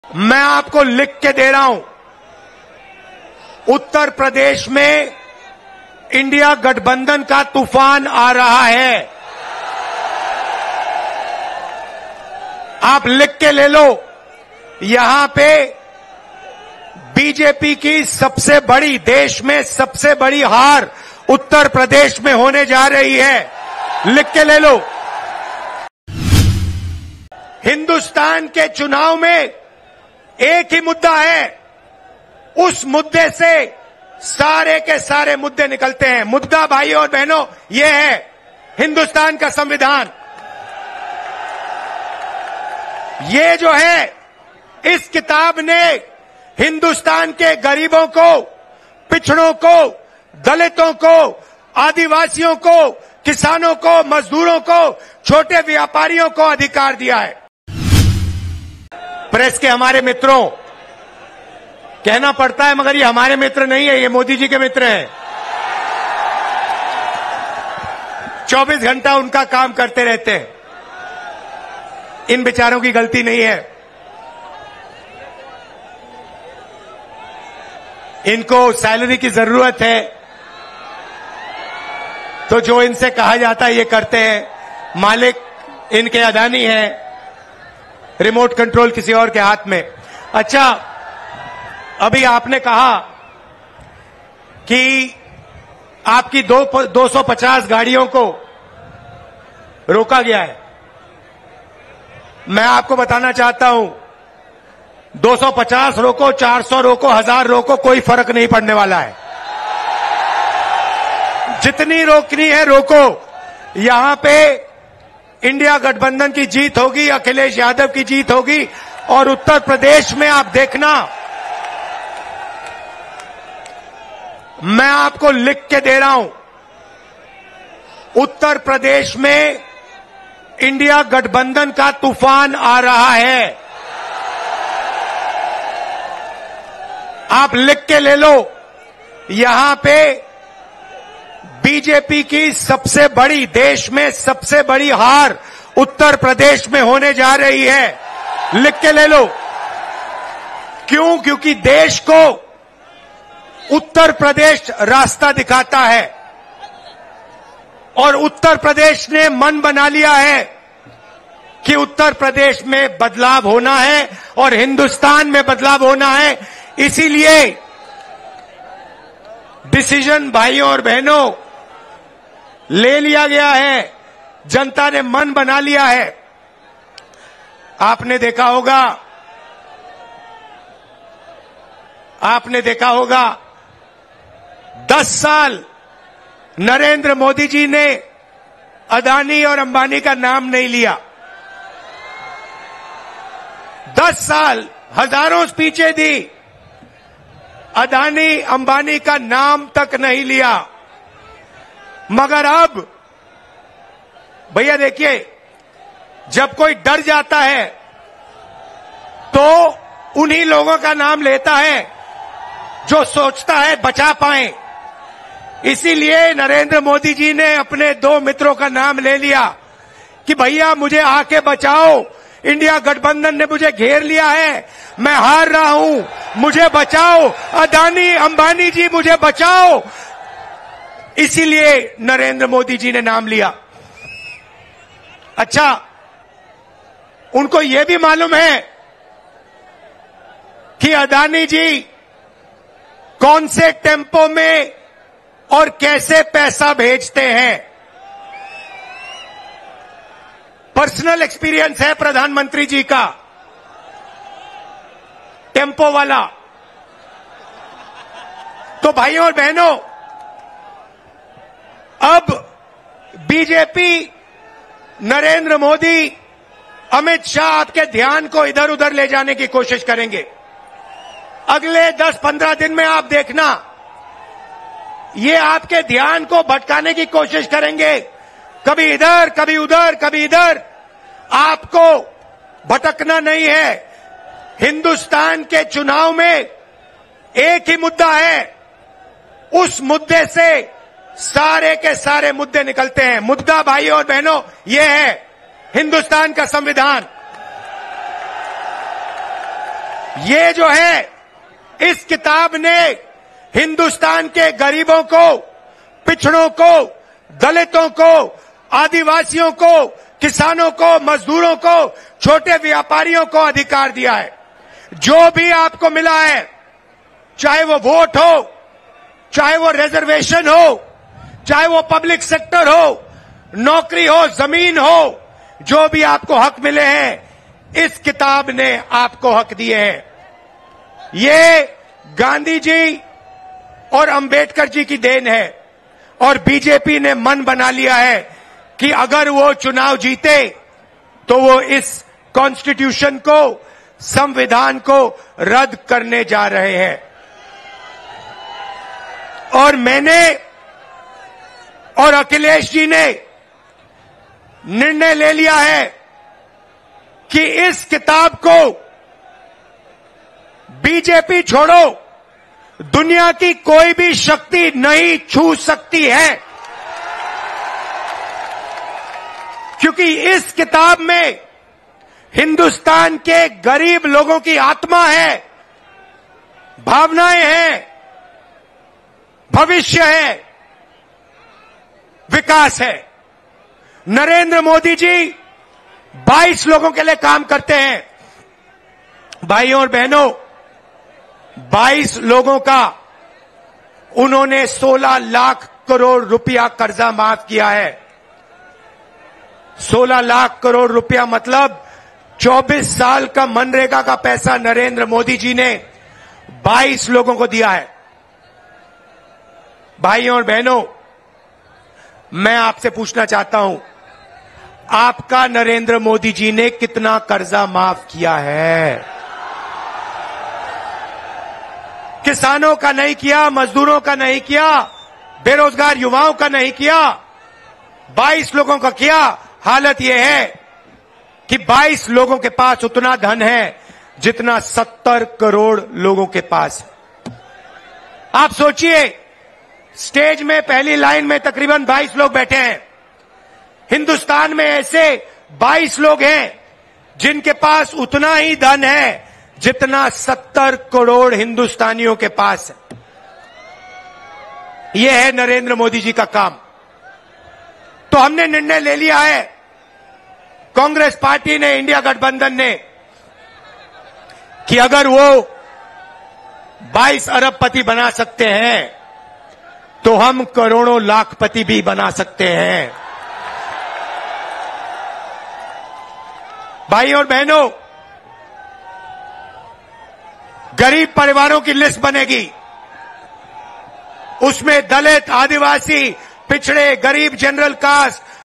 कन्नौज में राहुल गांधी ने योगी पर बोला ऐसा मंच पर बैठे अखिलेश भी सुनकर हैरान रह गए | News Patra
कन्नौज में राहुल गांधी ने योगी पर बोला ऐसा मंच पर बैठे अखिलेश भी सुनकर हैरान रह गए